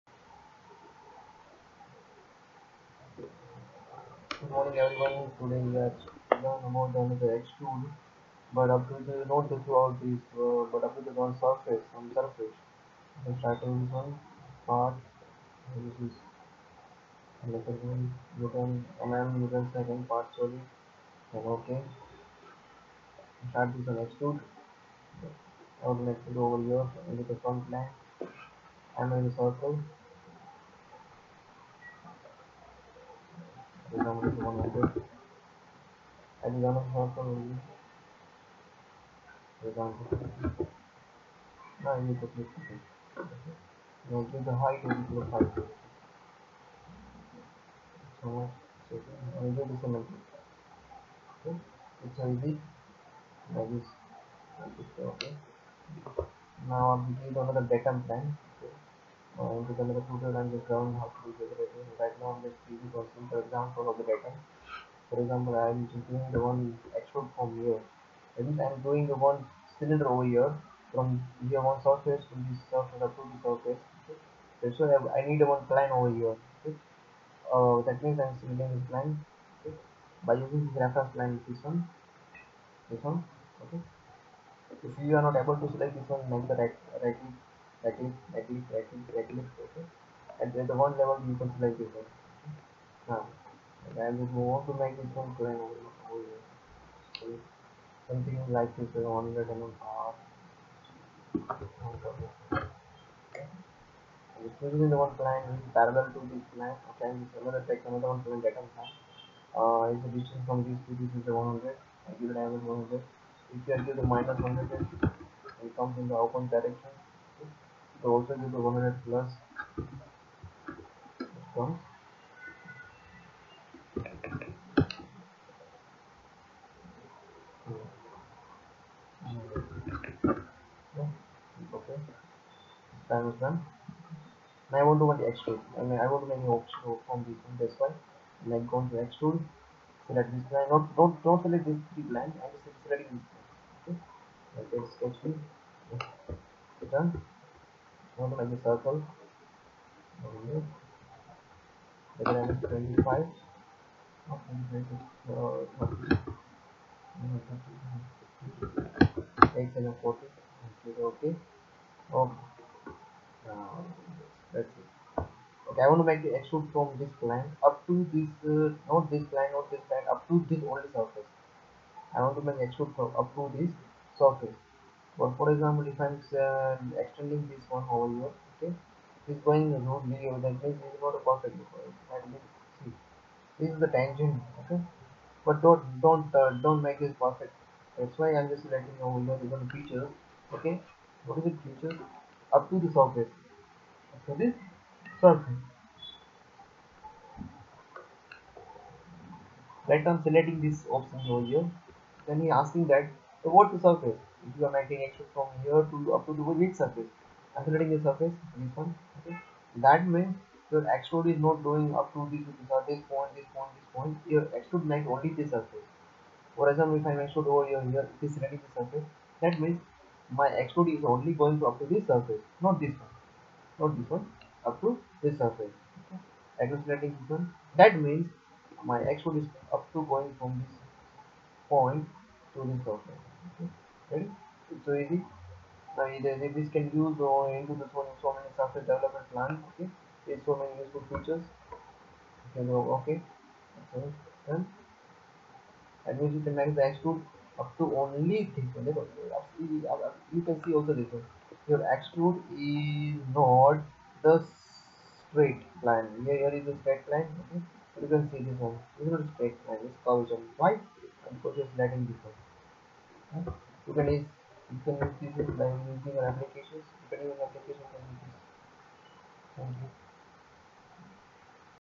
Creo 2.0: Extrude up to surface
into this tutorial, I gave the explanation about the extrude but up to the definite surface of the object.